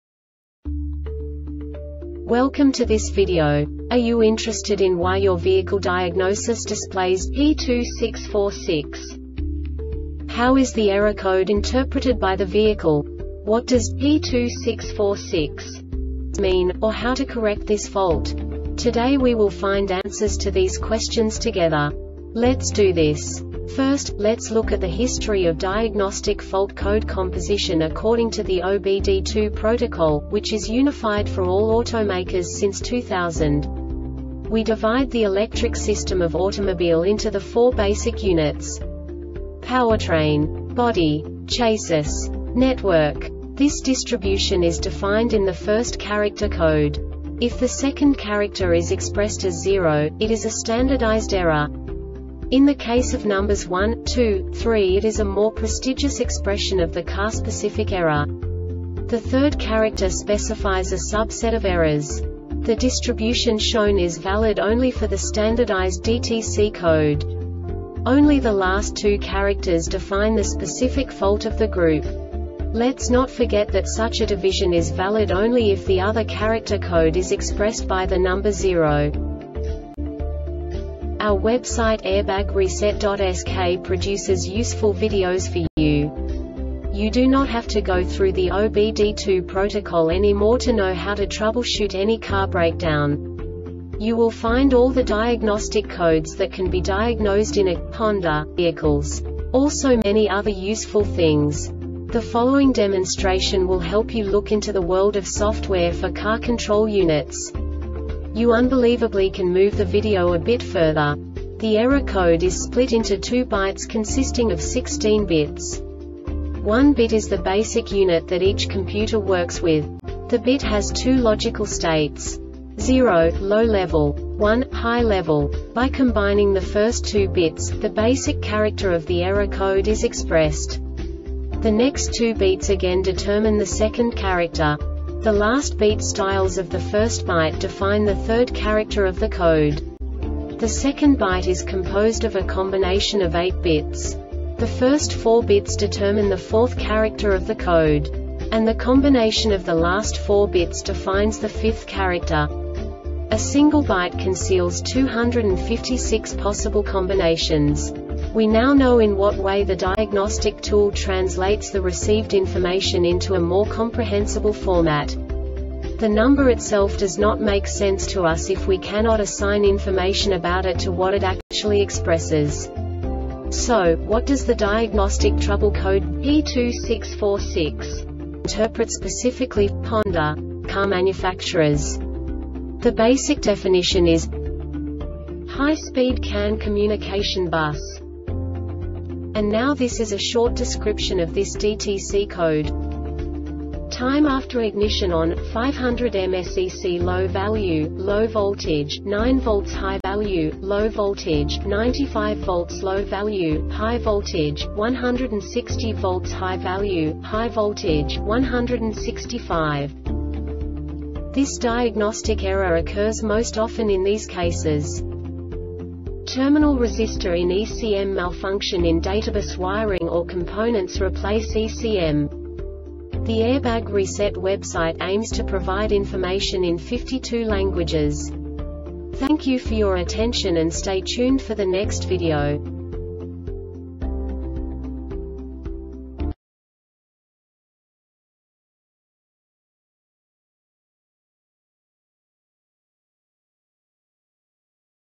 DTC Honda P2646 Short Explanation
Contents:
0:21 Basic DTC analysis according to OBD2 protocol standard.
1:48 Insight into programming
2:58 A diagnostically understandable description of the Diagnostic Trouble Code
3:05 Basic error definition

Make:
Honda
Code:
P2646
Definition:
High Speed CAN Communication Bus
Description: